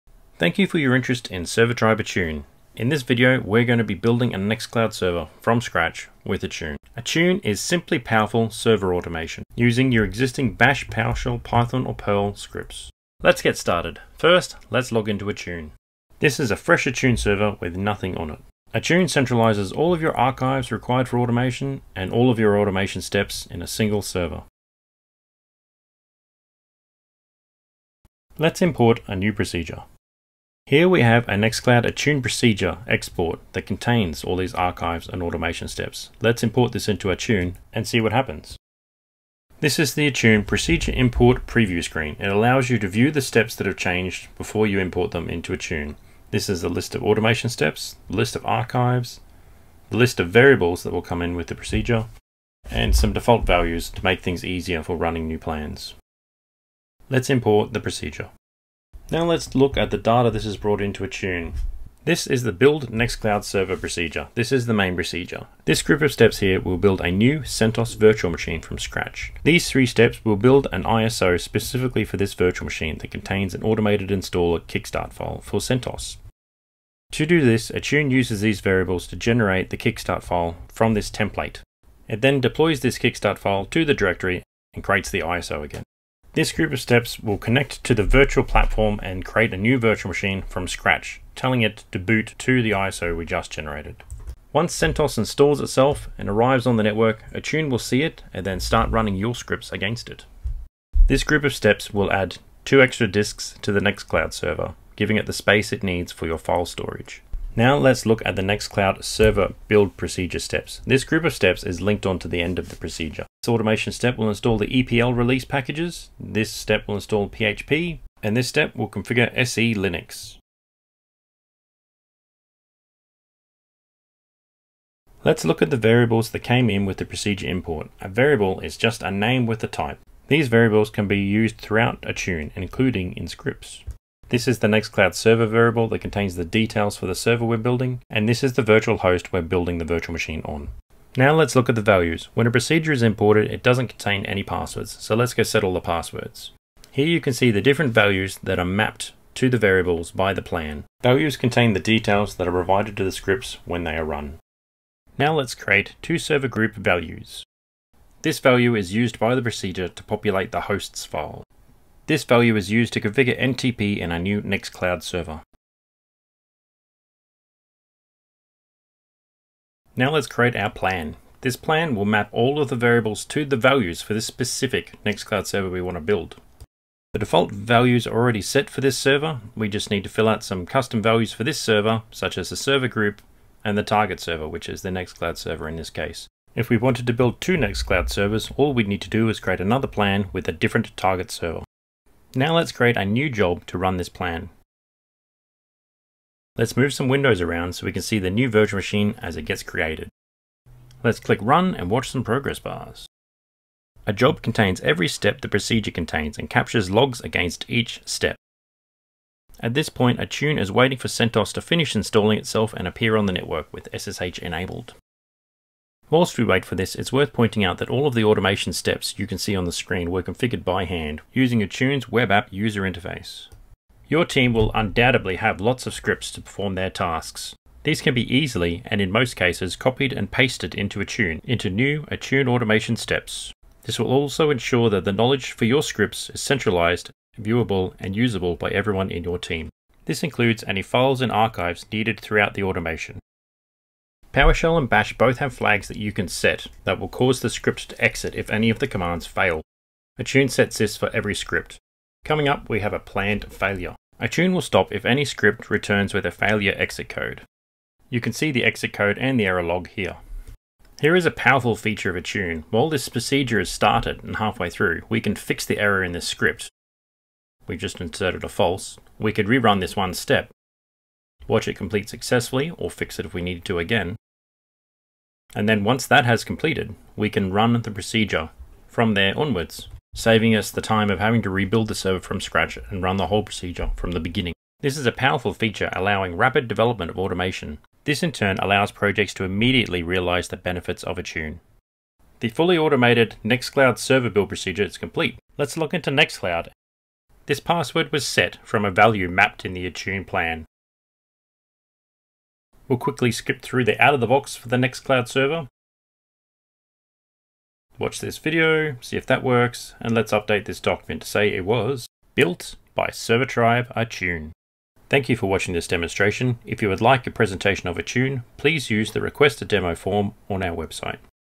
AttuneOps Server Automation Demo Building a NextCloud server
Use AttuneOps to build a NextCloud server with ease. Here is a short video on how we have used AttuneOps to build a Next Cloud server.

🔗 Resources